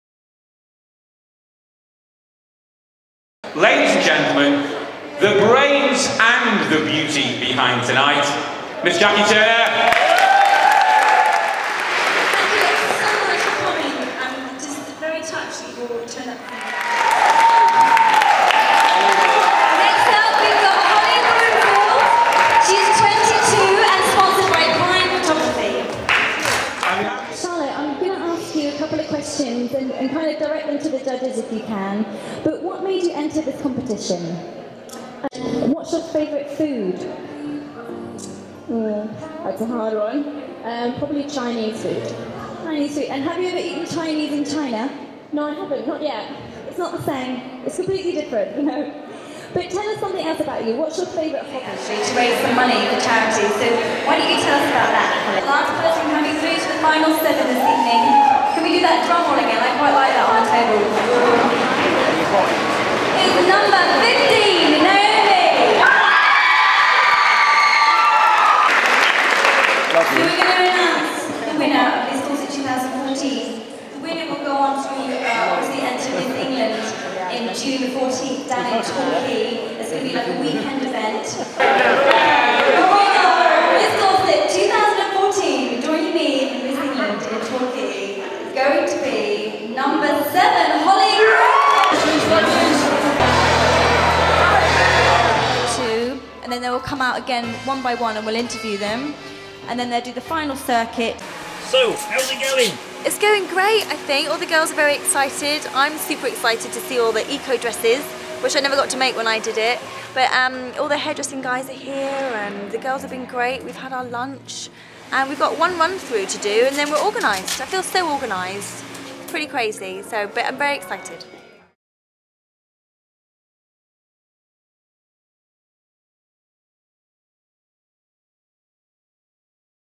Jackie Turner Presenting
Clips or Jackie presenting Miss Dorset 2014 at The Print Room